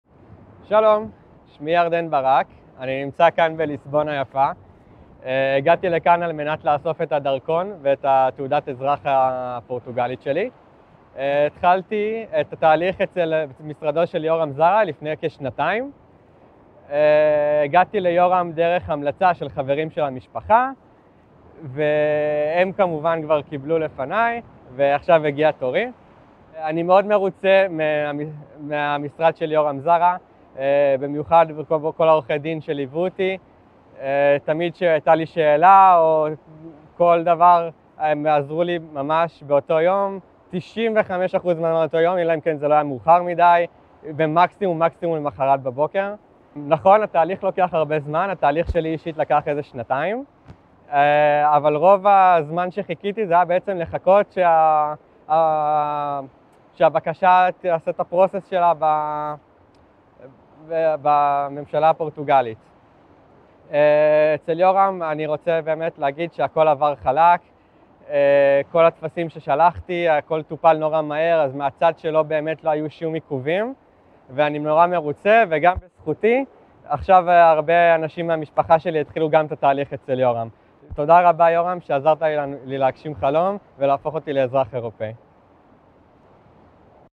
Yarden Barak
I came to Yoram through the recommendation of family friends. I’m very pleased with the Yoram Zara Law Firm ,any time I had a question they helped me the same day, everything went smoothly and was handled very quickly. Thank you so much Yoram for helping me fulfil a dream and becoming a European citizen.